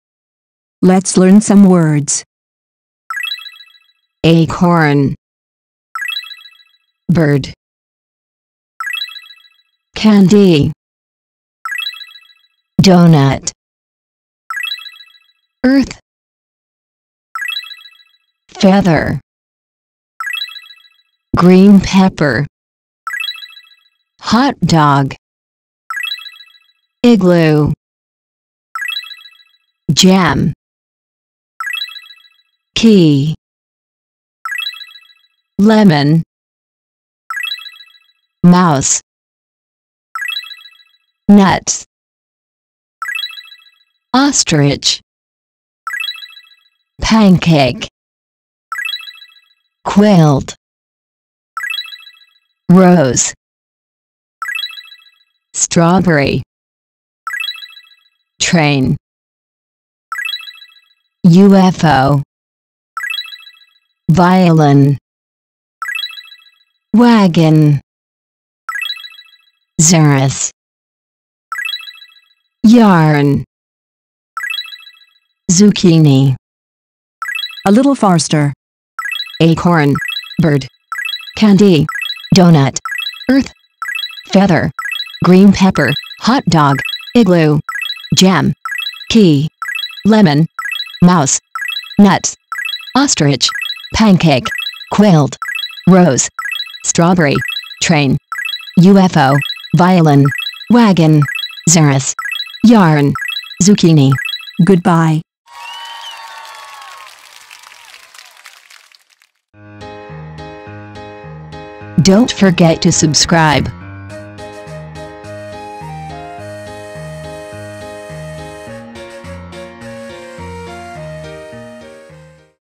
English [ABC words for young children 9] Flashcards Education For 0 to 3 years old
It is a flash card that remembers 26 kinds of words for young children.
You will naturally acquire English words along with the alphabets ABC to Z.

Flashcards activate the right brain by inputting a large amount of memory at once. By listening to the words while showing the picture on the card, you will develop the ability to process the image of the right brain and the ability to understand in the language of the left brain.
First ABC